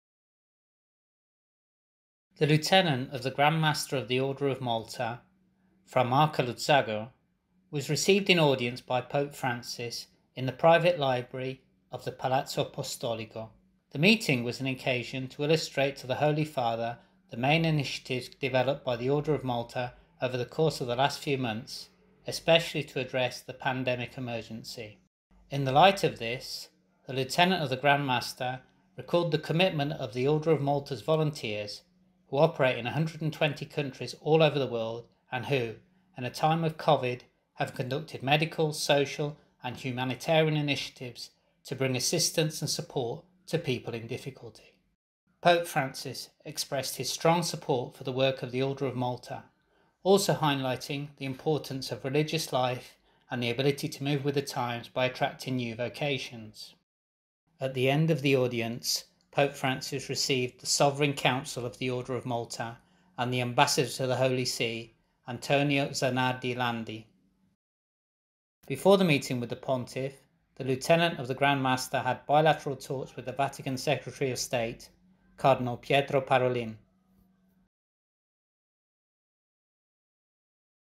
The Lieutenant of Grand Master Fra’ Marco Luzzago was received in audience by Pope Francis
Pope Francis receives in audience the Lieutenant of the Grand Master, Fra’ Marco Luzzago, in the Private Library of the Apostolic Palace. The meeting was an opportunity to illustrate to Pope Francis the main initiatives the Order of Malta has put in place in recent months, and in particular for tackling the pandemic.